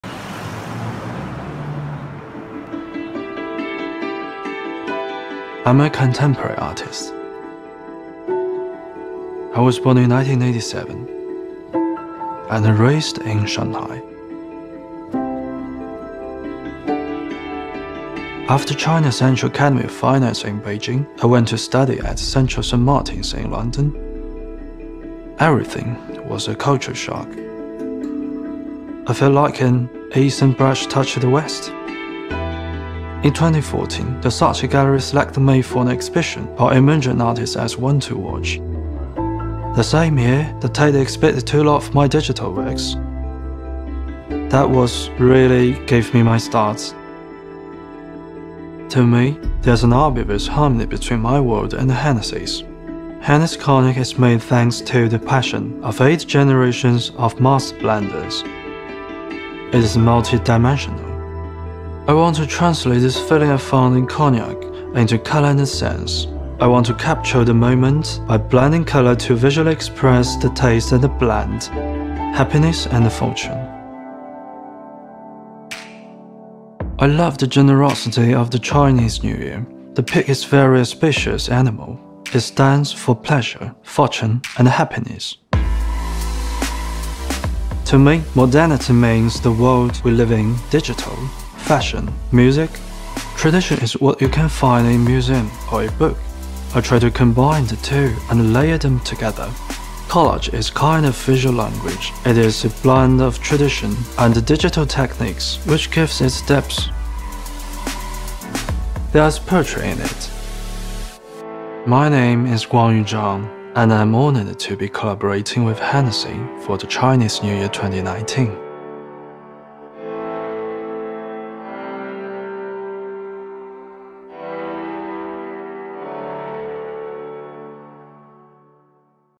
Hennessy X Zhang Guangyu - Art Collaboration - Chinese New Year 2019 limited edition - Artist video
The art of fusion embodied in a detailed and layered multidimensional artwork. To celebrate Chinese New Year, the artist Zhang Guangyu has created an incredible digital collage symbolizing the cyclical nature of life for the limited-edition Hennessy VSOP Privilege.

Client : @Hennessy
Artist : Zhang Guangyu
Agency : @SOMEXING